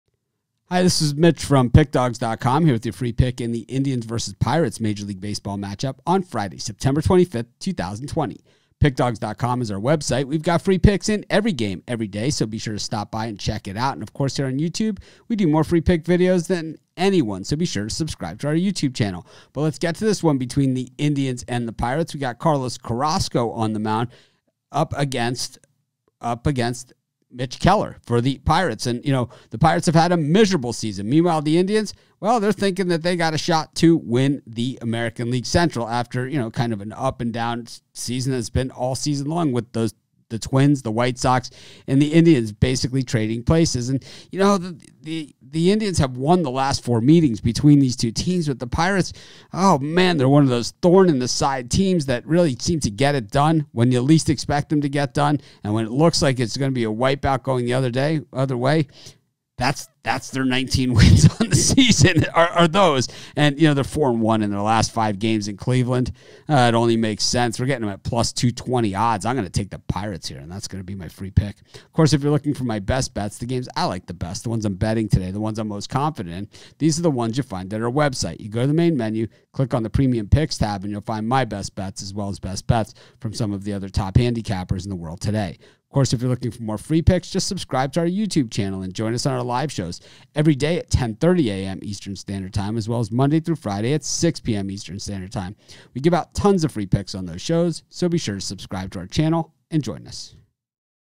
Cleveland Indians vs Pittsburgh Pirates Free Pick 9/25/20 MLB Pick and Prediction MLB Tips
The Pittsburgh Pirates face off with the Cleveland Indians in Friday's MLB Baseball action. Get Mitch's Expert Pick and Prediction for this game.